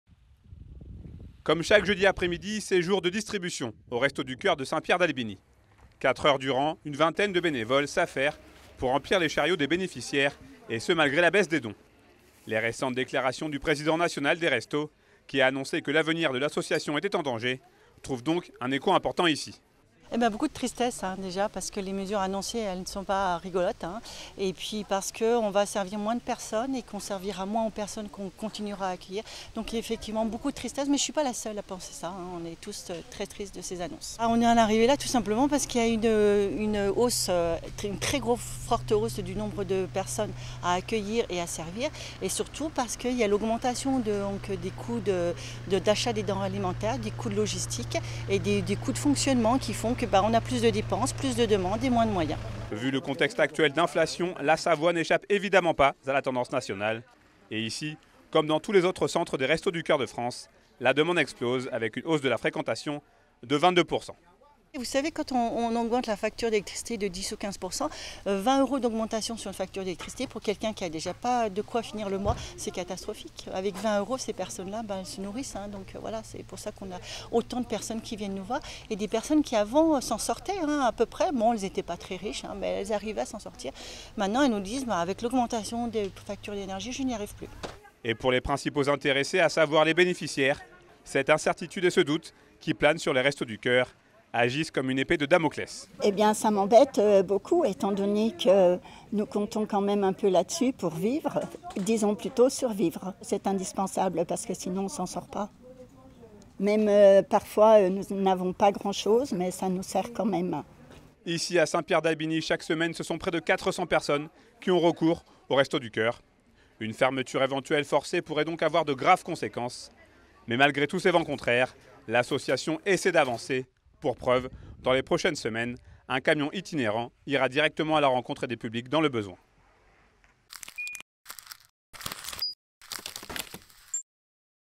Inquiétude aux Restos du Coeur
Direction Saint-Pierre d’Albigny où le centre local des Restos du Coeur doit faire à une demande toujours plus importante de bénéficiaires. Malheureusement, cette hausse de la fréquentation s’accompagne d’une baisse des dons et d’une vraie inflation des prix. Une fermeture éventuelle des Restos serait donc une catastrophe pour beaucoup de personnes dans le besoin.
Sébastien Germain s'est rendu sur place.